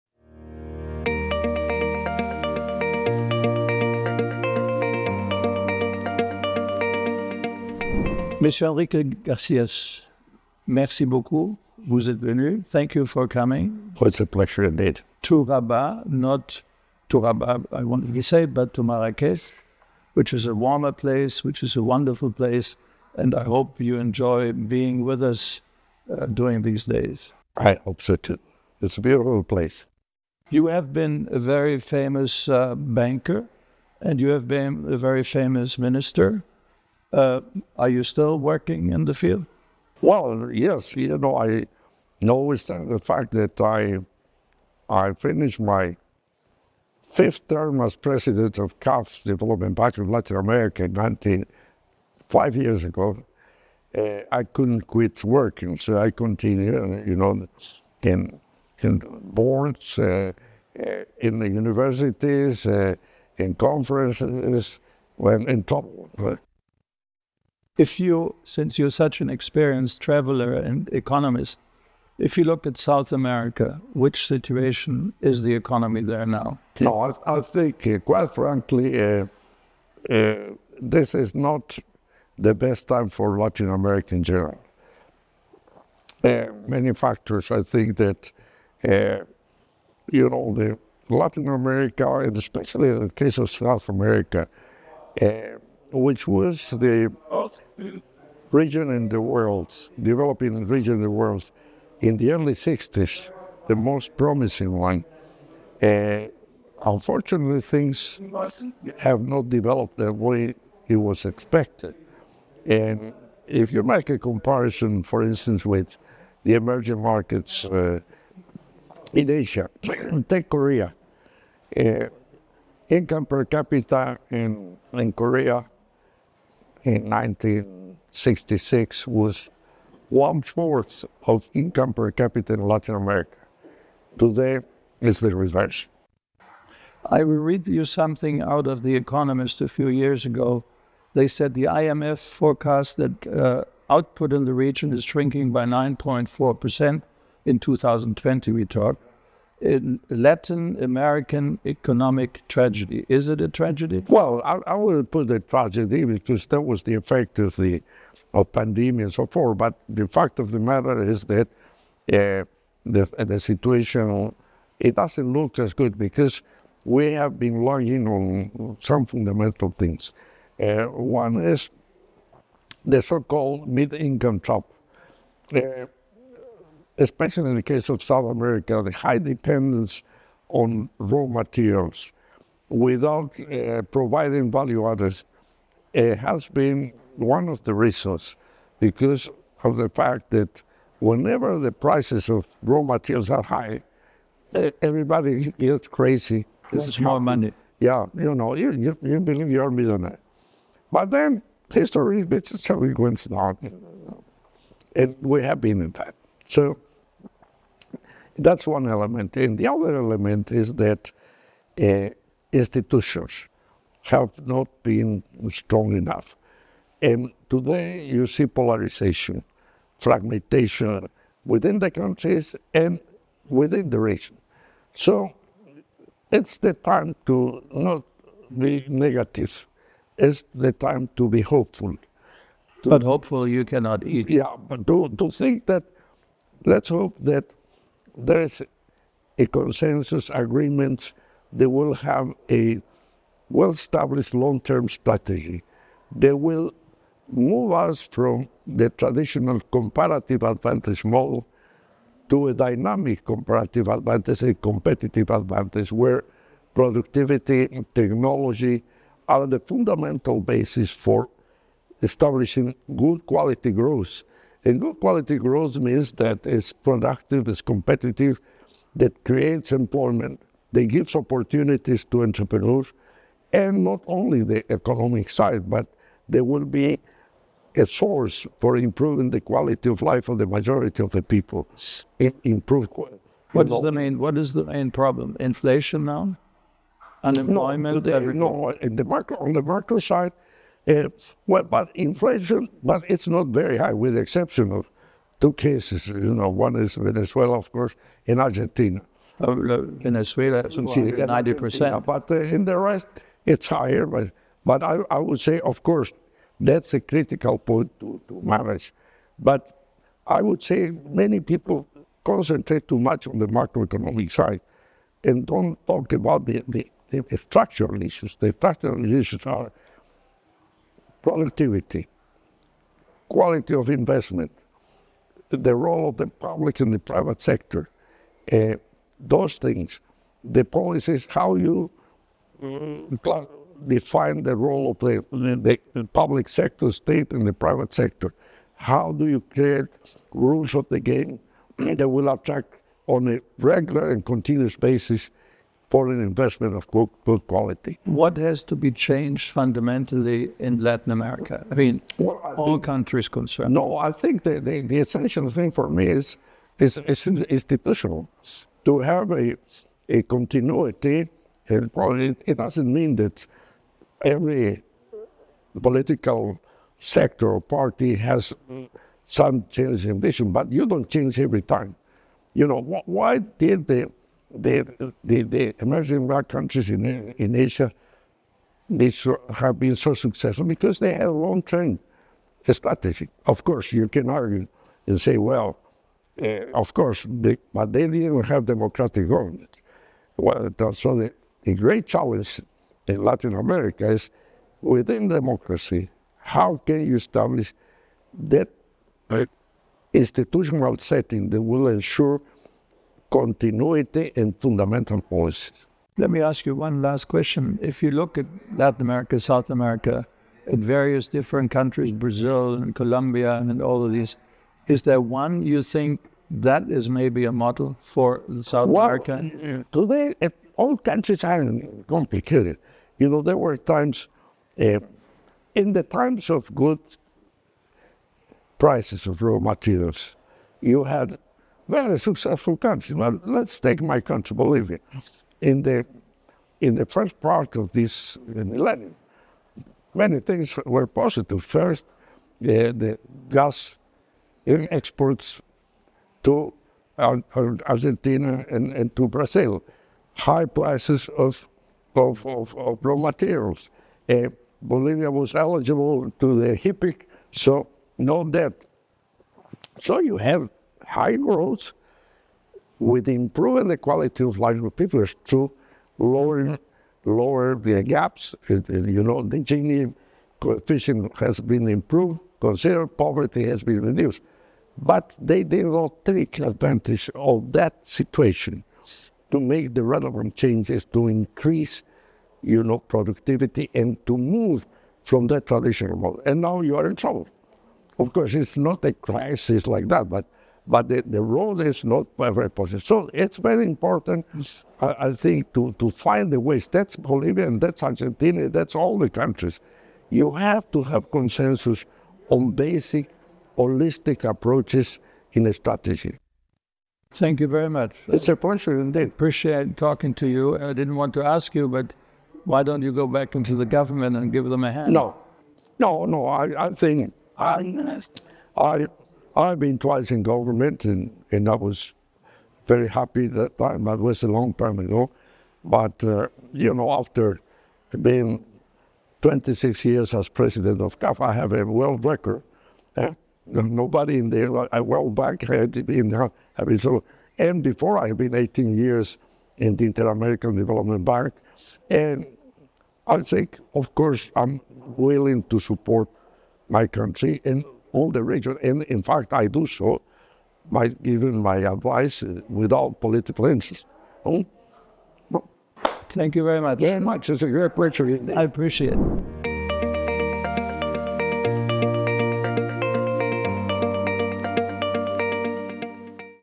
Assessing Economic Decline in South American Countries
In this podcast, Enrico Garcia, former President and CEO of the CAF Development Bank of Latin America and a distinguished figure in Bolivian governance, analyzes the intricate factors behind South America's profound economic downturn. Drawing on his extensive experience in both the public and financial sectors, Mr. Garcia shares insights into multifaceted issues including economic policies, political dynamics, and external influences.